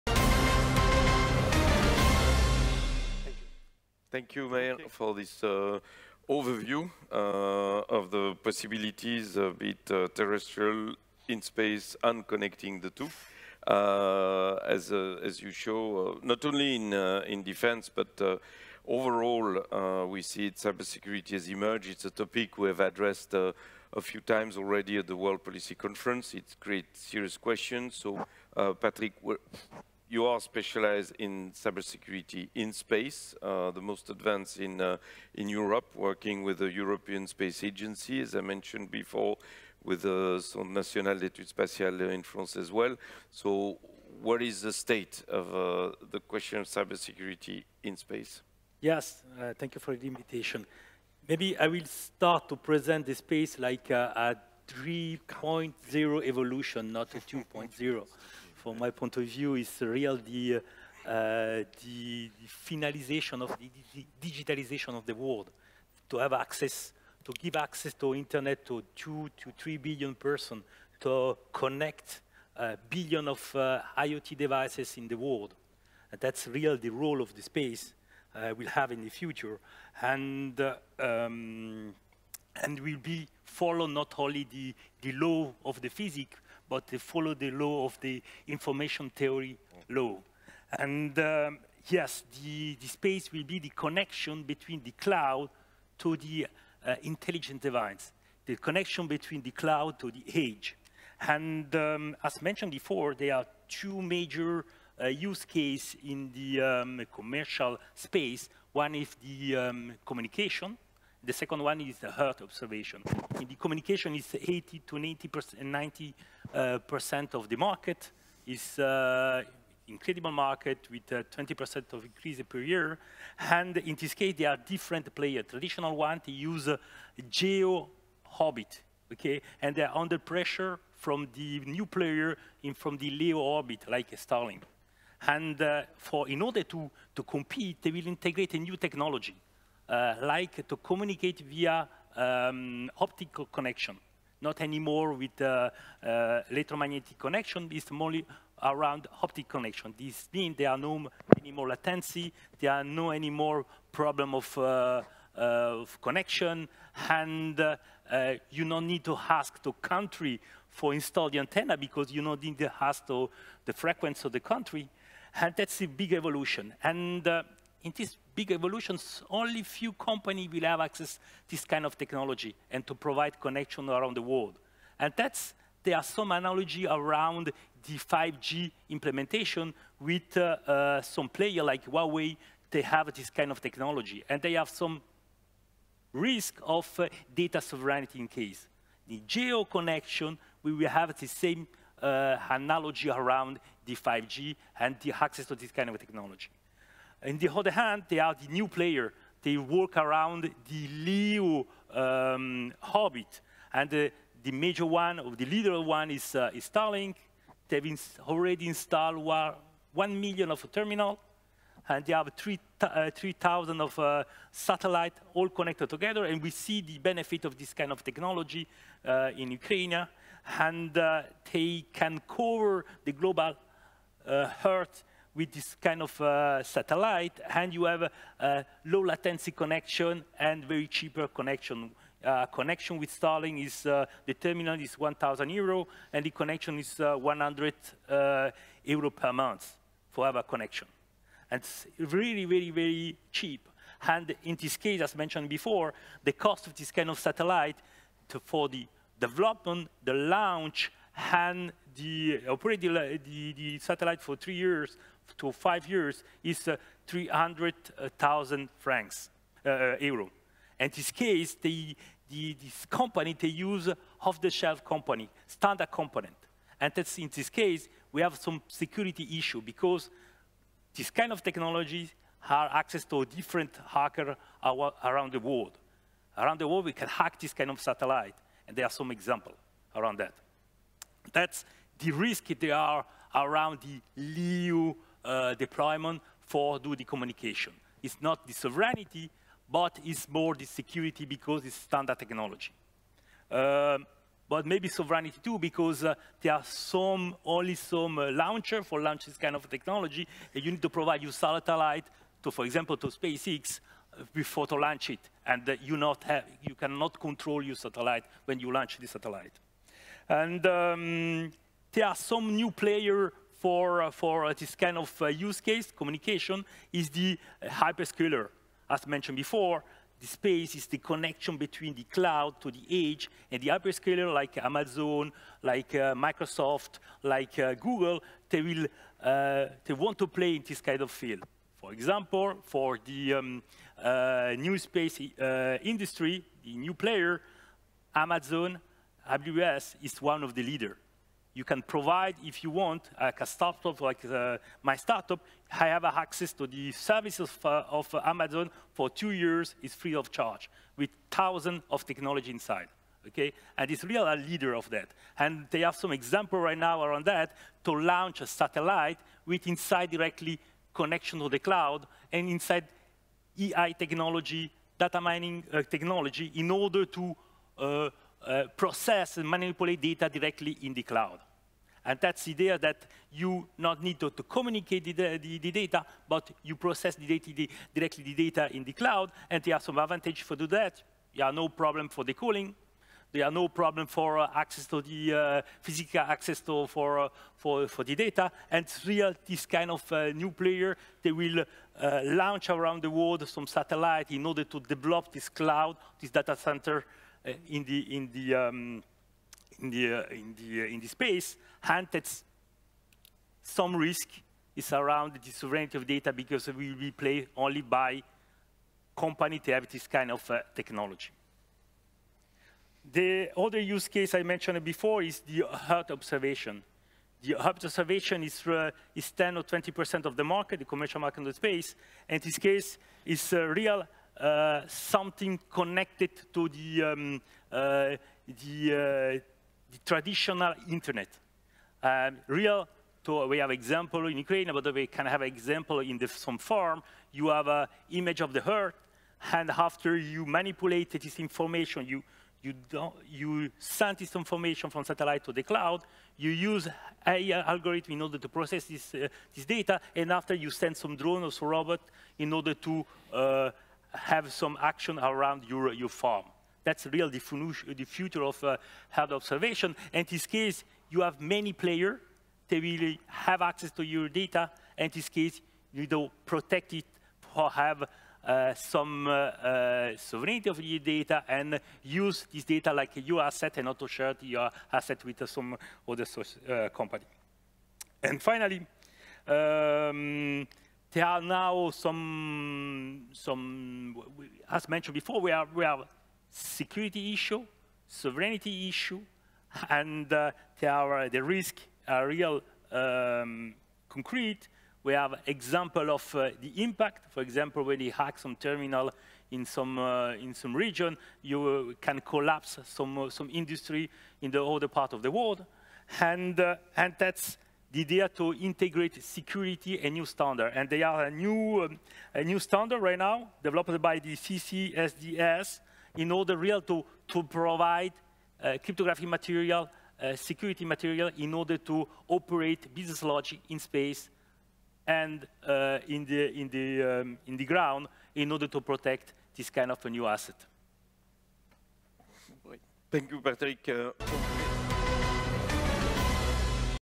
WPC 2022 – Patrick Trinkler – Session 9 – Space Governance
Session 9 – Space Governance: The Implications of Globalized Access to Space Technologies

Patrick Trinkler is the Founder and CEO of CYSEC